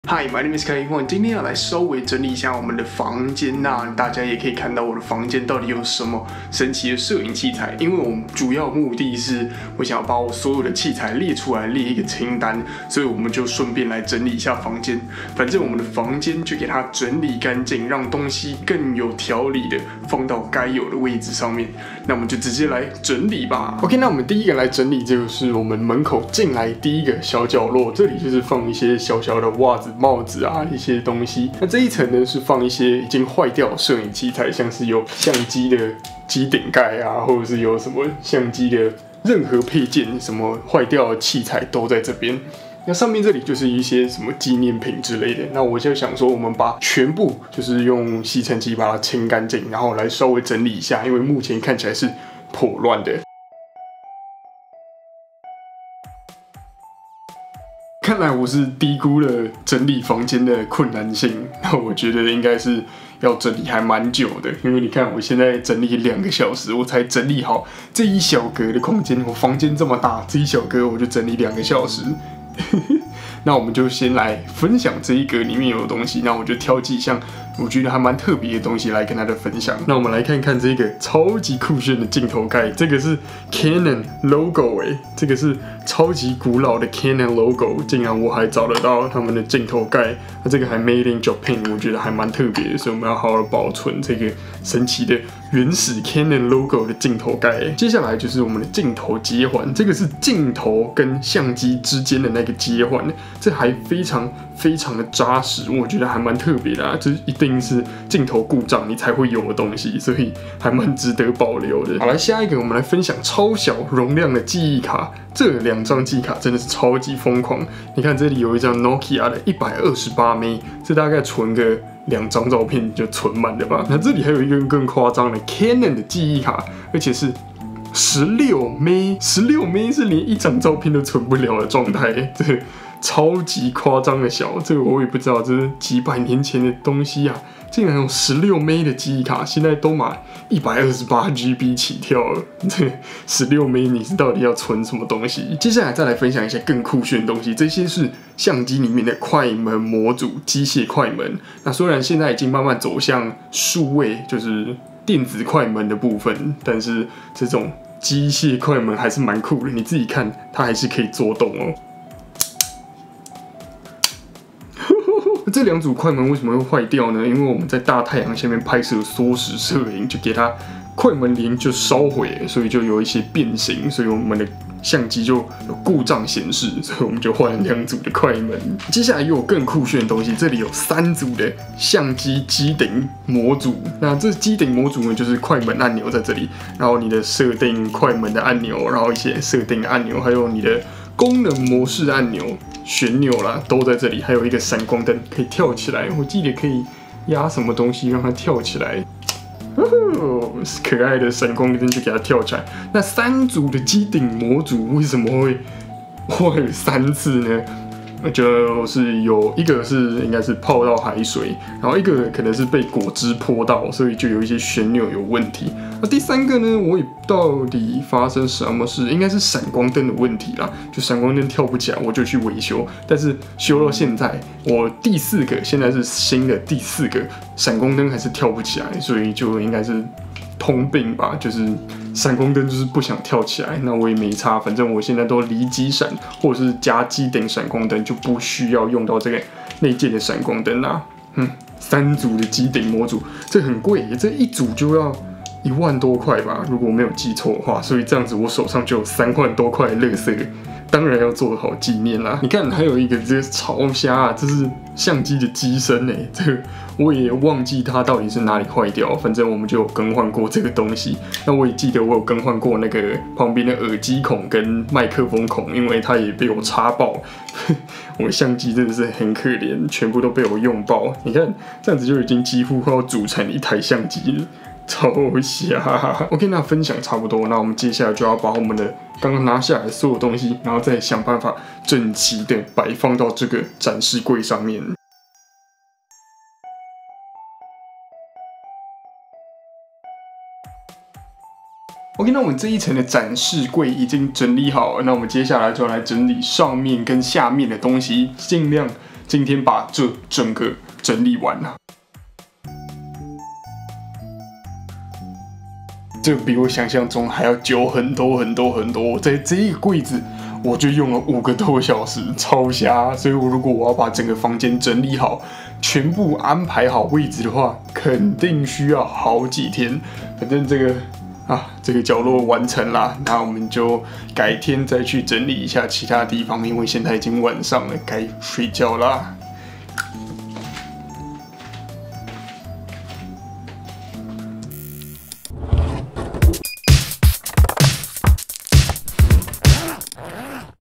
攝影師房間大公開 稀有器材收藏 百萬攝影師房間到底有甚麼? Part.1 小看整理房間的困難性
百萬攝影師房間到底有甚麼? Part.1 小看整理房間的困難性